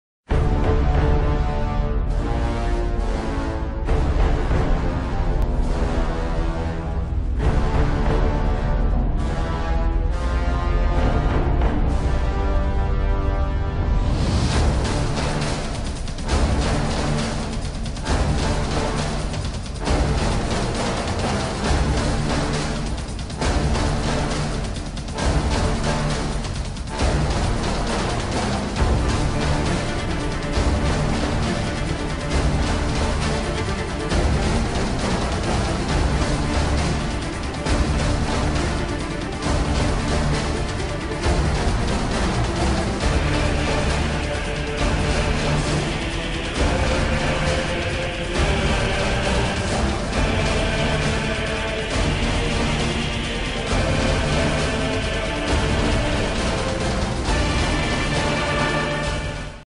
Blue and White Game: West Florida Football
White defeats Blue 24-6